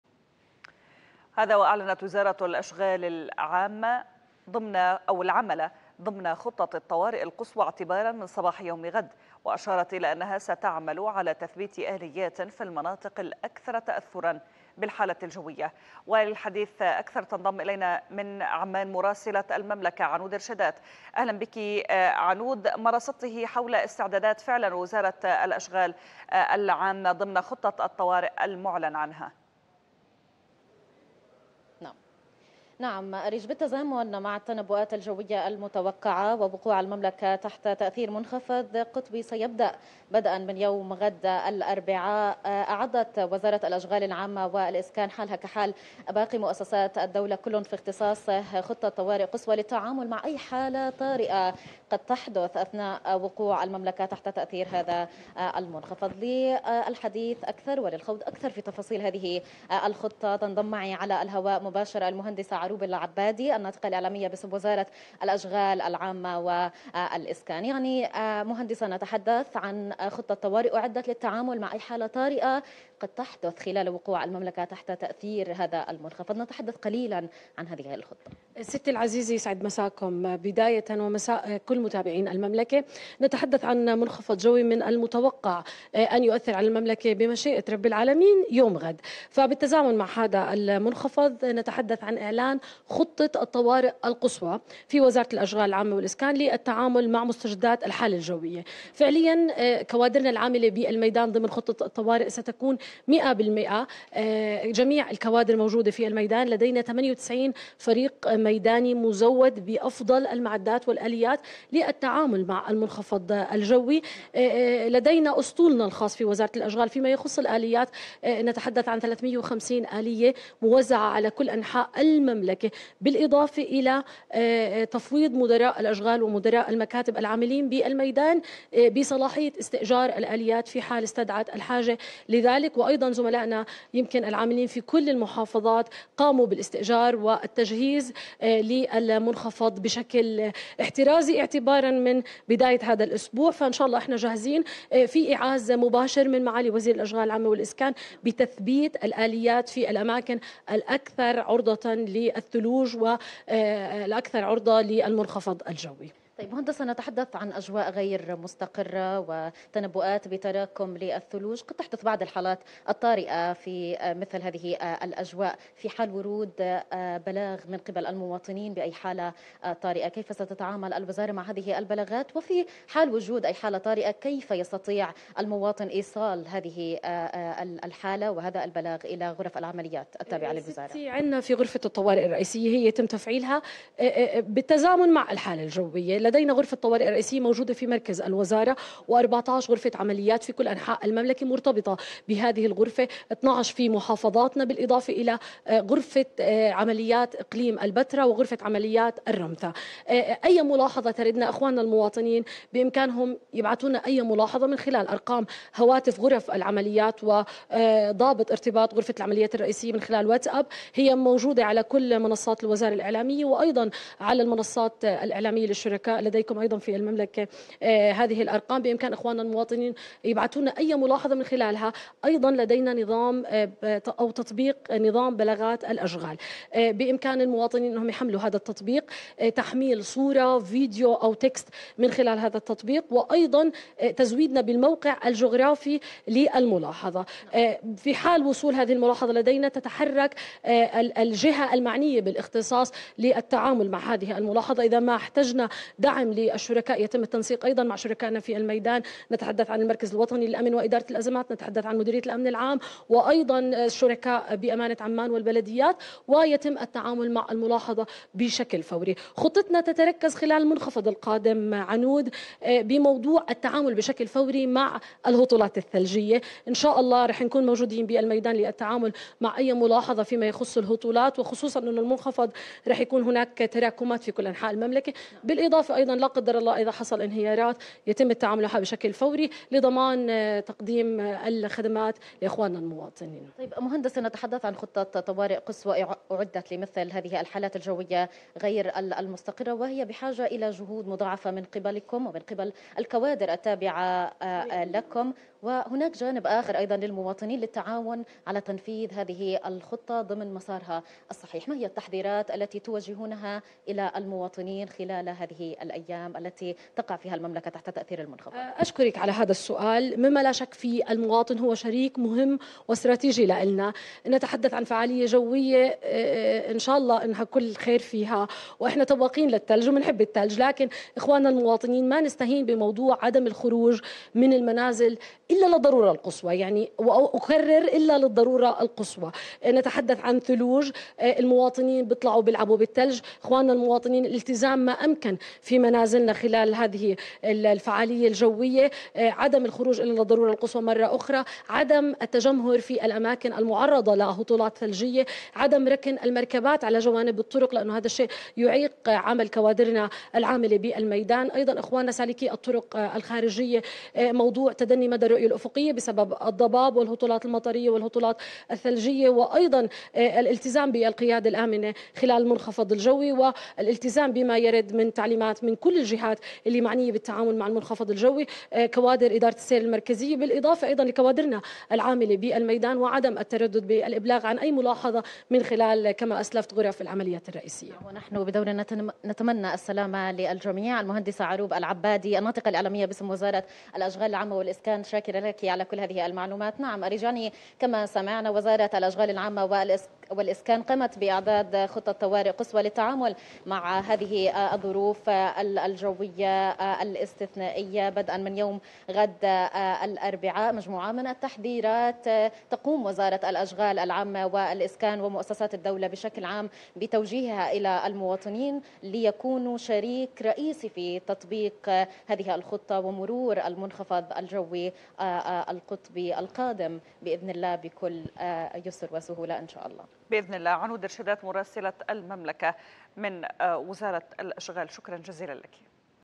وزارة الأشغال: تم تثبيت آلياتنا في المناطق الأكثر تأثرا بالمنخفض
مراسلة المملكة عنود ارشيدات